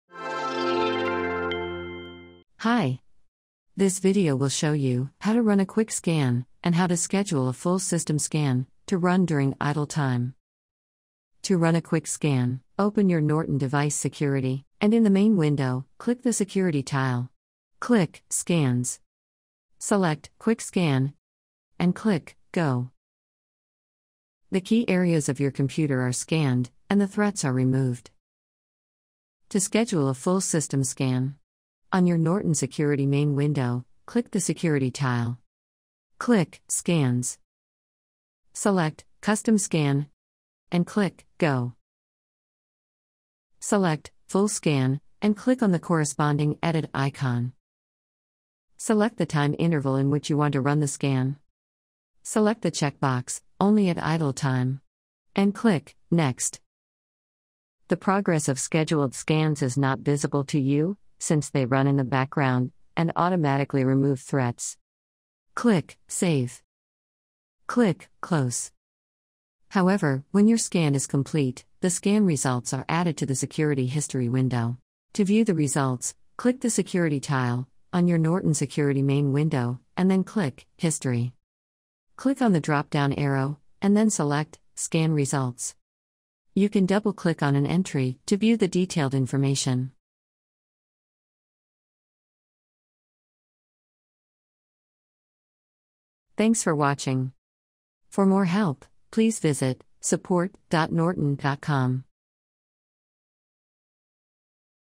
How to Run a Scan on Norton Protection for Viruses, Spyware, and Security Threats
This video shows you how to run a scan in Norton to detect and remove viruses, spyware, and other security threats from your computer.

A Full System Scan thoroughly examines your entire computer for viruses, spyware, and other security threats. It usually takes an hour or even a day depending on the number of files in your computer.